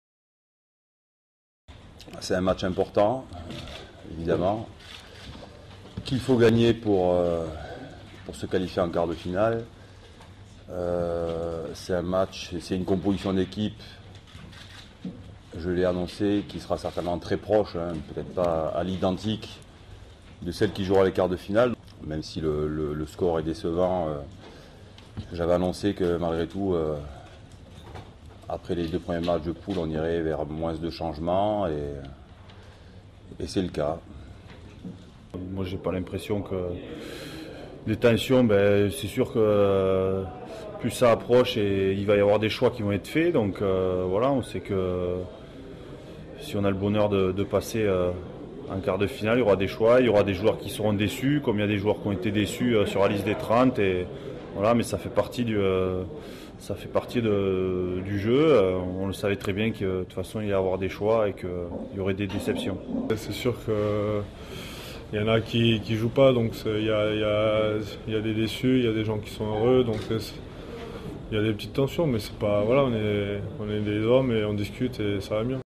Conférence de presse de l'équipe de France de Rugby à Auckland
L'entraineur Marc Lievremont et les joueurs de l'équipe de France de rugby se sont exprimé pendant la conférence de presse pour l'annonce de la constitution de l'équipe pour le match de la coupe du monde de rugby 2011 contre les Tonga. Conférence de presse. Durée: 01:15.